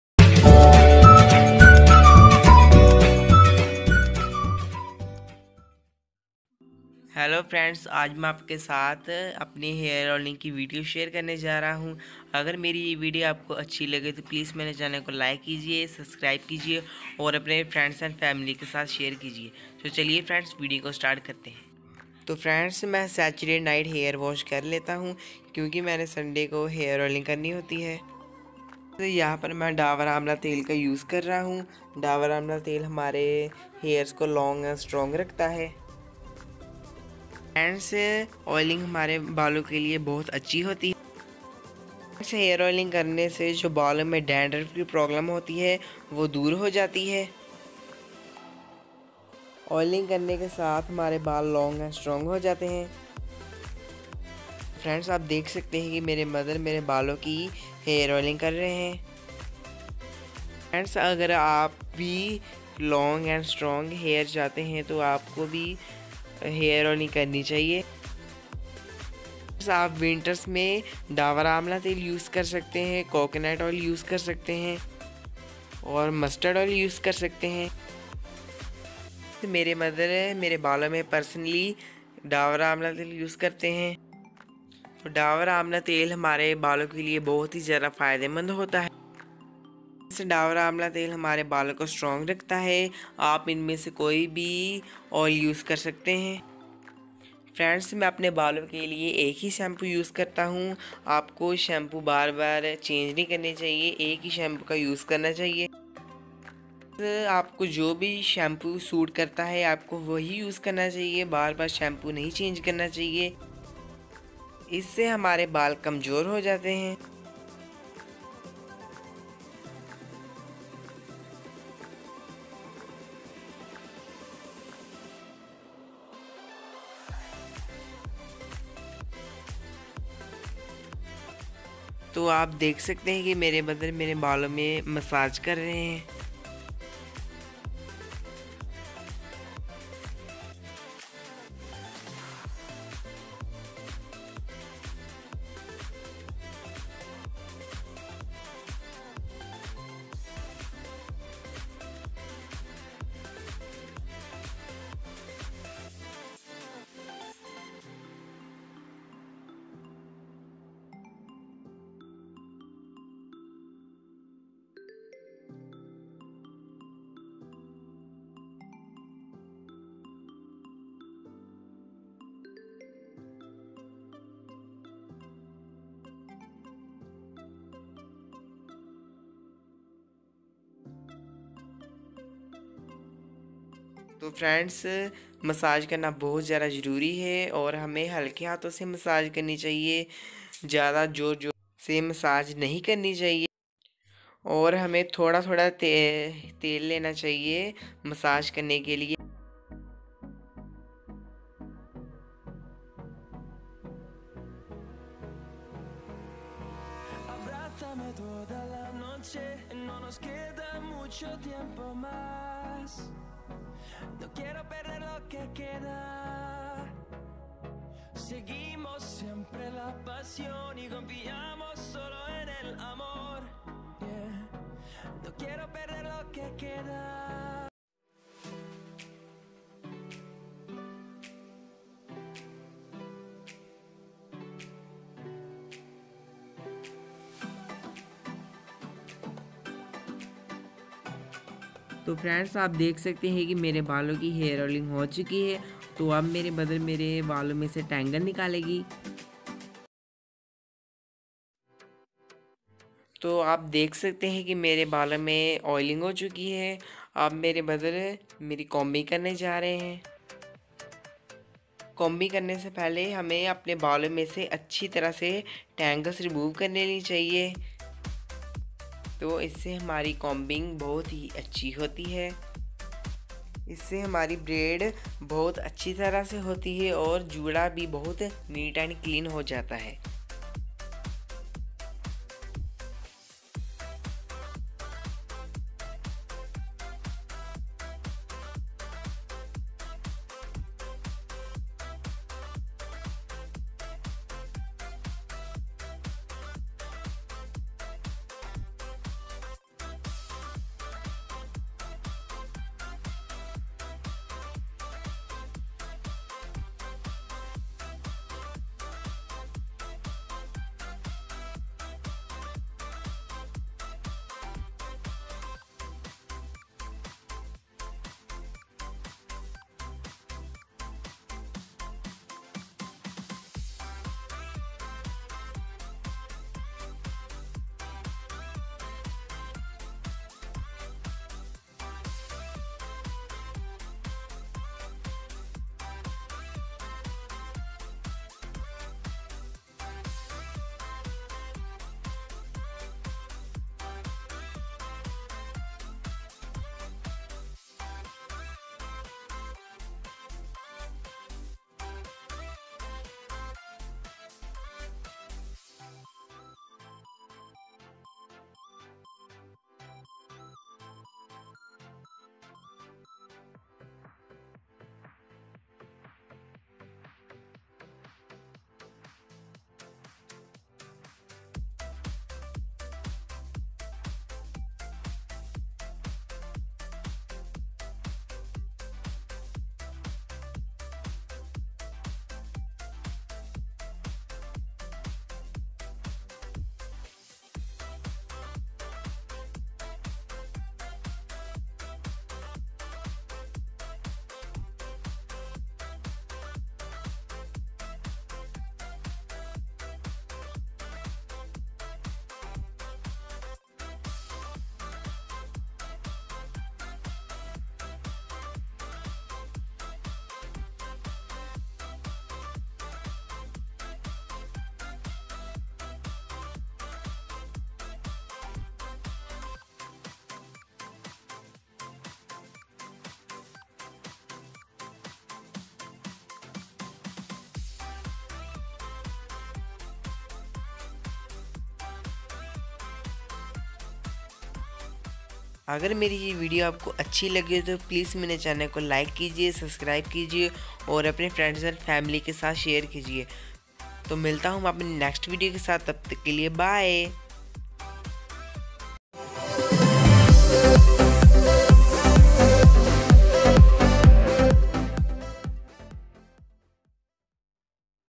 Heavy Hair Oiling With Dabour Amla Hair Oil Very Long Strong Healthy Hair
With
Amla oil
Healthy hair
Strong hair
Thanku
Friends